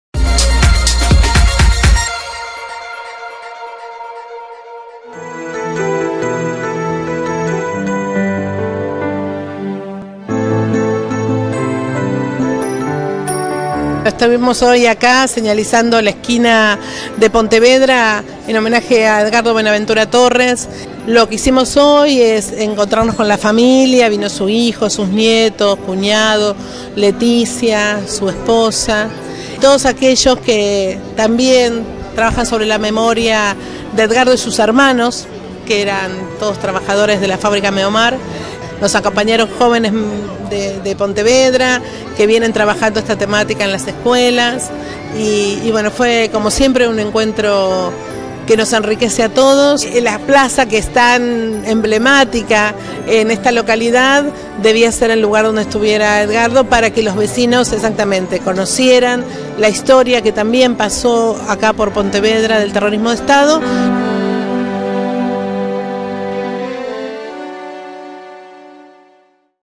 Esquinas de la Memoria: Homenaje a Edgardo Buenaventura Torres
La Dirección de Derechos Humanos de Merlo realizó la vigésima Esquina de la Memoria,en homenaje a Edgardo Buenaventura Torres, secuestrado-desaparecido el 08/12/1976 por el terrorismo de estado.